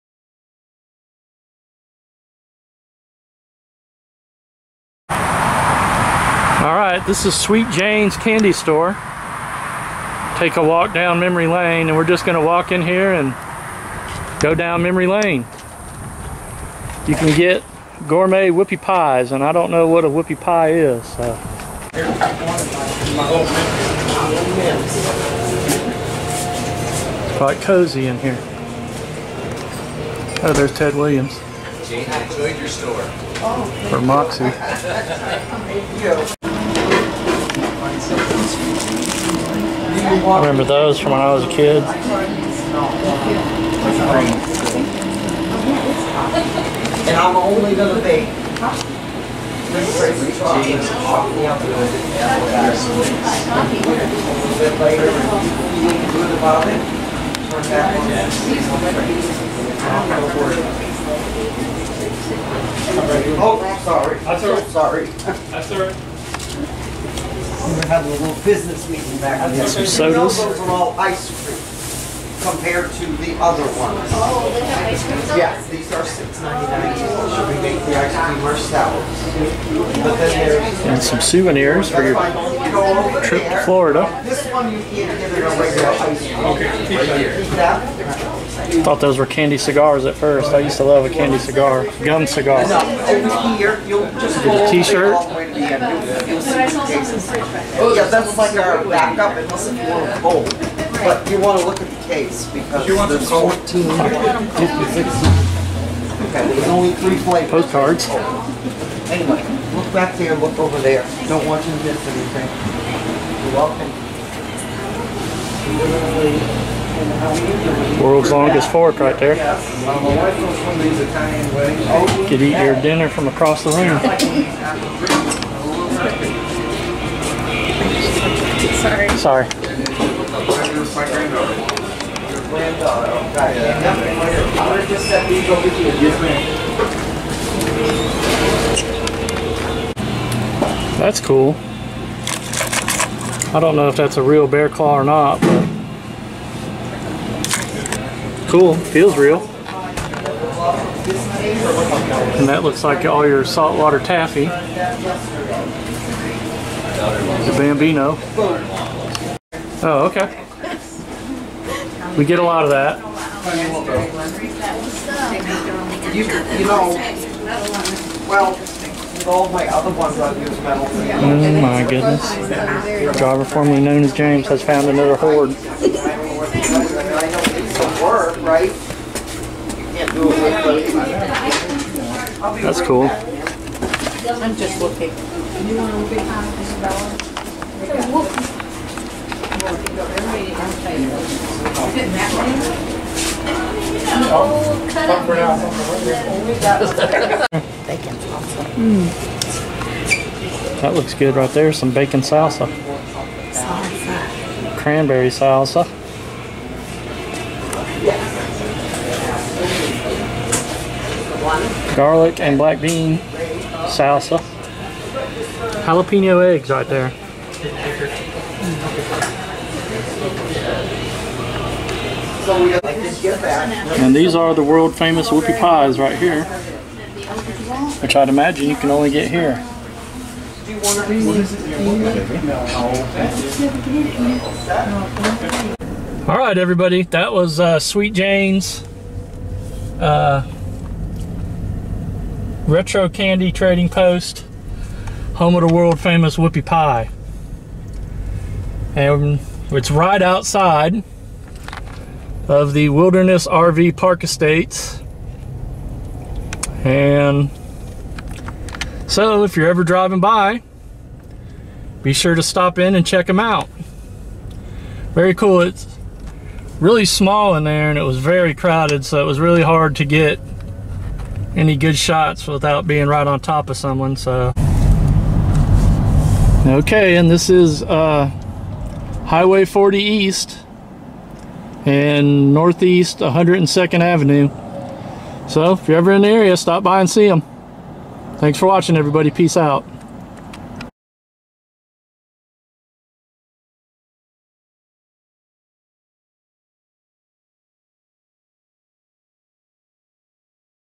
Sweet Jane's Candy Store & Trading Post | Silver Springs, Florida | Retro Candy & Whoopie Pies.
A very cool and cozy retro candy shop located a few miles outside of Ocala Fl. Stop in and get a Whoopie Pie!